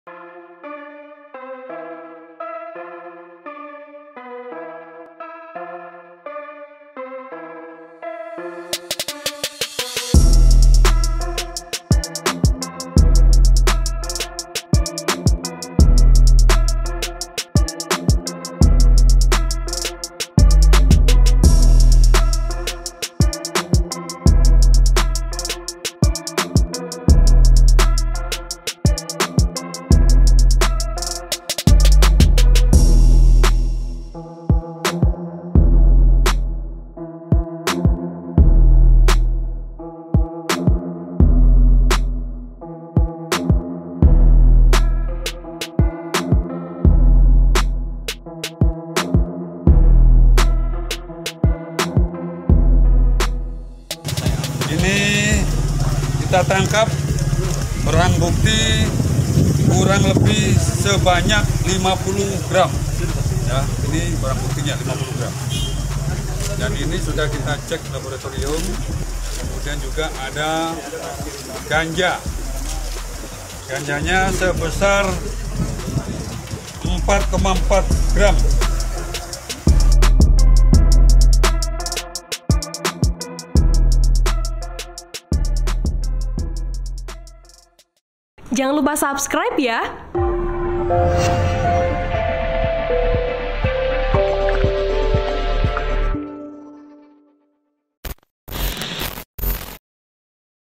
Polres Palu Tangkap 7 Pelaku Tindak Pidana Narkotika, Miliki Uang Tunai Rp210 Juta
TRIBUN-VIDEO.COM - Kepolisian Resor Palu kembali menangkap sebanyak 7 orang terduga pelaku tindak pidana narkotika.

Masing-masing berinisial DH (25), FA (38), HY (24), HD (29). SR (30), SH (32), dan AS (33).

"Dari ketujuh tersangka ini, ada yang memiliki uang tunai sebanyak Rp210 jita, dan kita akma kembangkan apakah terkait tindak pencucian uang atau tidak," jelas Kapolres Palu AKBP H Moch Sholeh, Kamis (16/1/2020) sore.
Mereka ditangkap di 5 tempat kejadian perkara (TKP) berbeda.

Untuk motifnya kata Kapolres Sholeh, para terduga pelaku itu mengaku karena faktor ekonomi dan adanya kesempatan.

Namun tegas Kapolres Sholeh, pihaknya masih mendalami kasus tindak pidana narkotika tersebut.

Adapun barang bukti yang berhasil diamankan polisi, yakni alat hisap sabu, sejumlah smartphone dan kendaraan bermotor roda dua.

Dari semua tersangka polisi menyita sabu - sabu seberat 52,99 gram bruto dan ganja 4,4 gram bruto.

Berdasarkan hal itu, para tersangka dikenai pasal 114 ayat 1 subsidair pasal 112 ayat 1 UU RI nomor 35 tahun 2009 tentang Narkotika, dengan ancaman hukuman 5 - 12 tahun penjara.